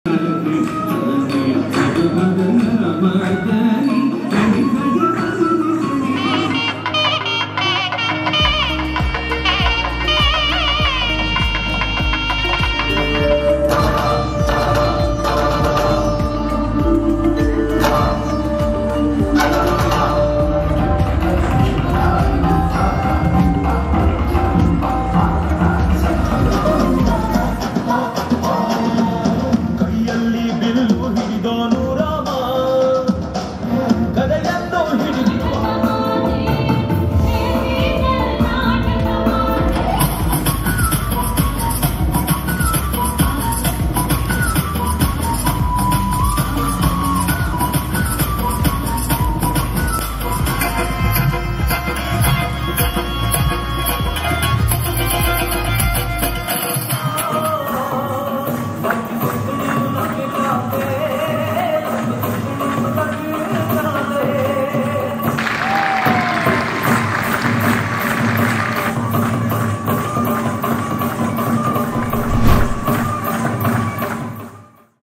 LG of Delhi celebrated the Foundation Day of of 8 States & 5 UTs at Talkatora Stadium
Hon'ble Lieutenant Governor of Delhi Shri Vinai Kumar Saxena celebrated the Foundation Day of of 8 States & 5 UTs at Talkatora Stadium along with their residents in the Capital, amidst enthralling cultural performances that livened the spirit of Ek Bharat Shreshtha Bharat in the Capital.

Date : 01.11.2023